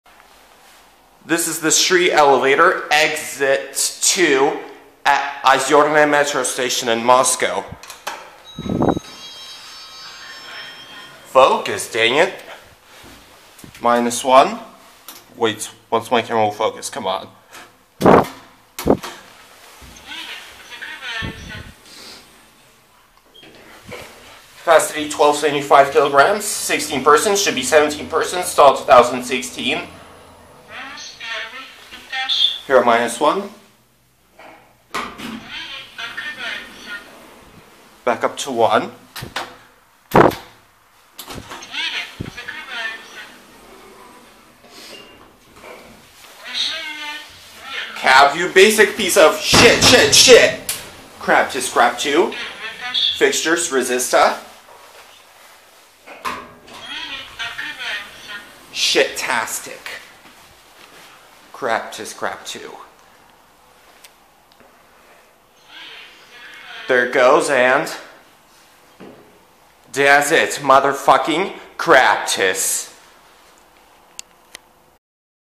BRAND NEW Otis Gen2 MRL Traction Elev. at Ozёrnaya Metro Station, Troparёvo-Nikulino in Moscow, R
Typical Craptis a hunk of shit.

It started it's service this month.

SCREW YOU GORMOST, OZЁRNAYA METRO STATION AND CRAPTIS FOR INSTALLING THIS PIECE OF CRAP (PIECE OF CRAP ELEVATORS)!!!

This station opened: 30th August 2018

---Exit 2 Street elevator---
Brand: Otis (Craptis)
Type: MRL Traction
Capacity: 1275kg / 16 (should be 17) Persons
Model: Gen2 (Crap2)
Fixtures: Resista
Installed at 2016
Floor served: 2(-1, *1*)
Ministry of Industry, Trade & Labour number: E2NI9639